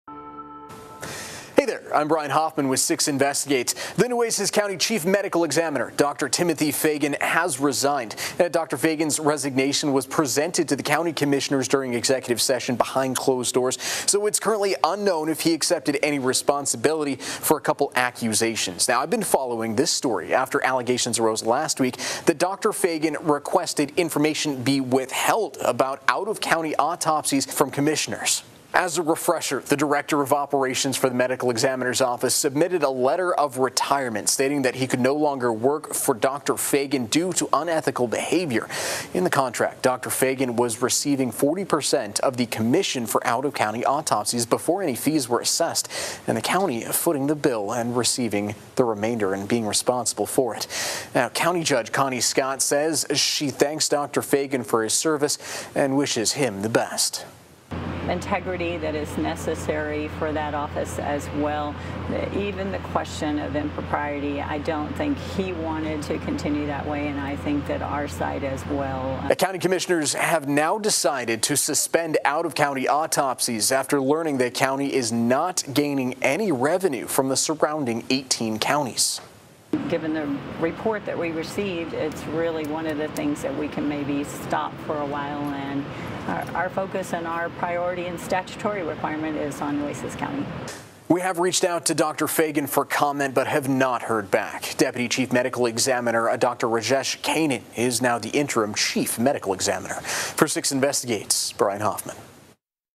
Nueces County Medical Examiner resigns
Nueces County Medical Examiner Dr. Timothy Fagen has resigned. This resignation comes one week after accusations of "unethical" behavior.